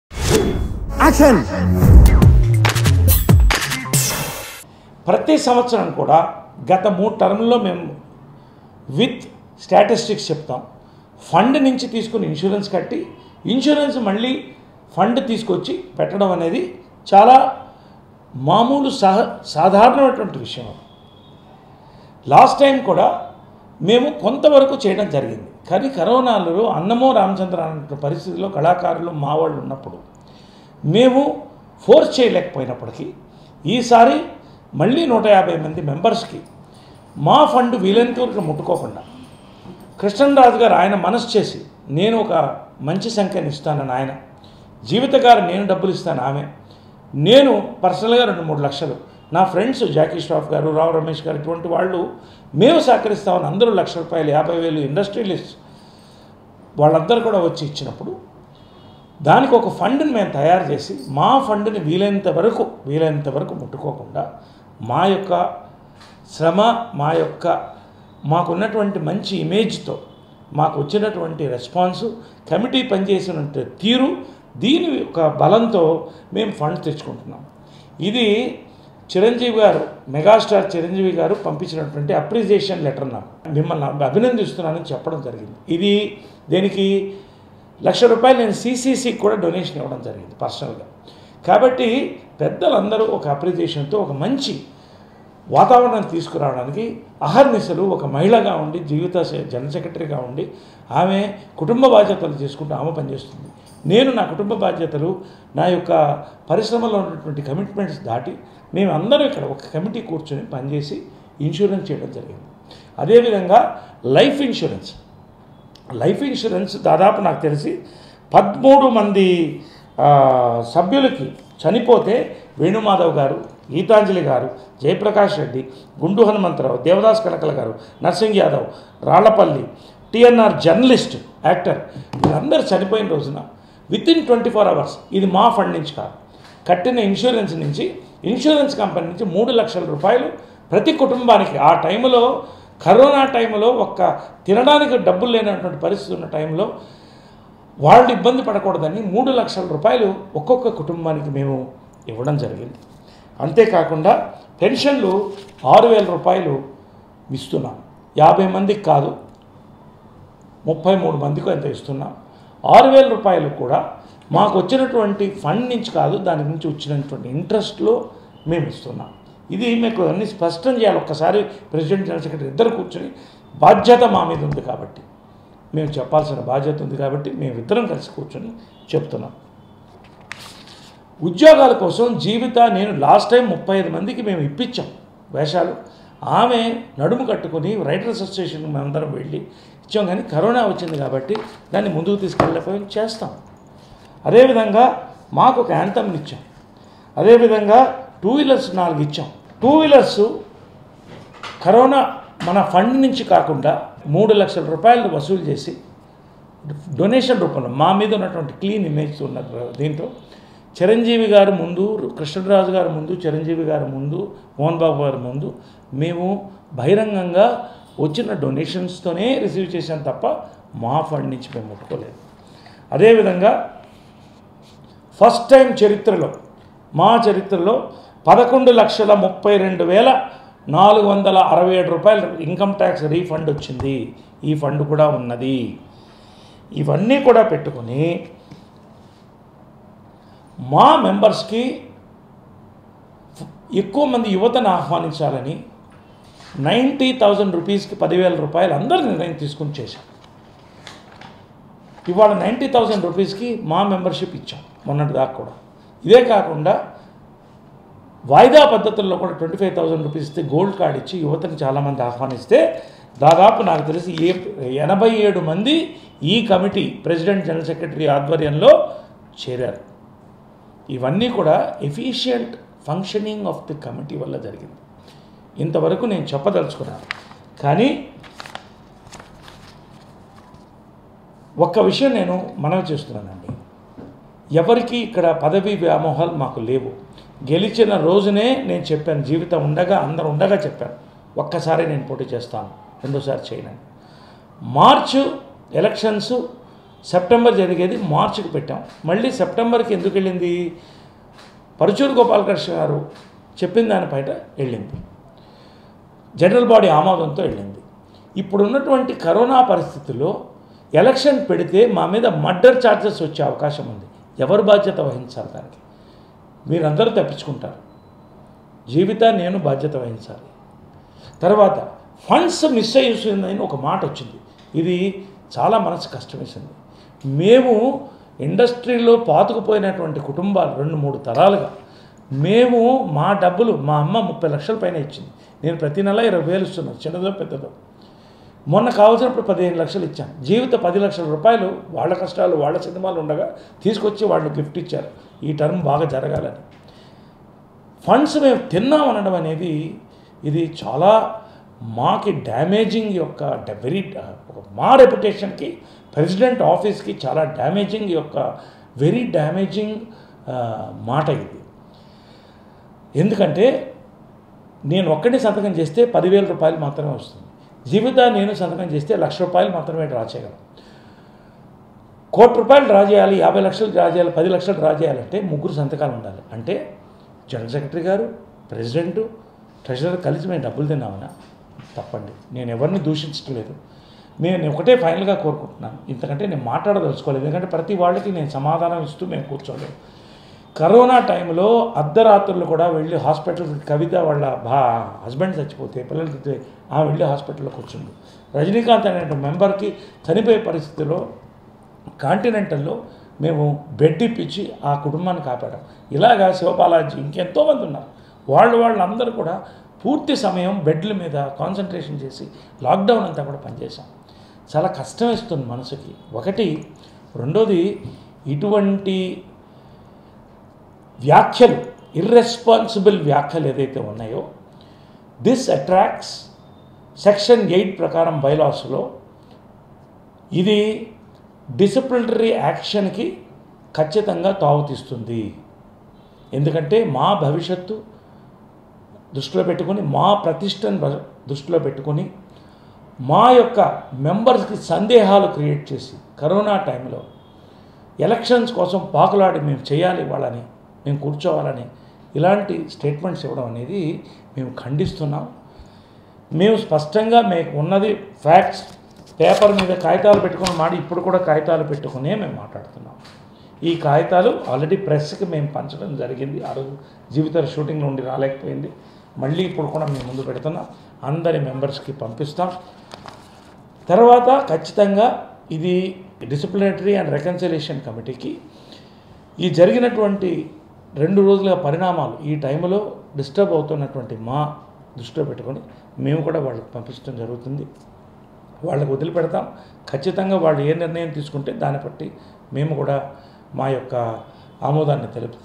MAA President Naresh Leaked Chiranjeevi Mobile Message | Maa Elections 2021
MAA President Naresh Leaked Chiranjeevi Mobile Message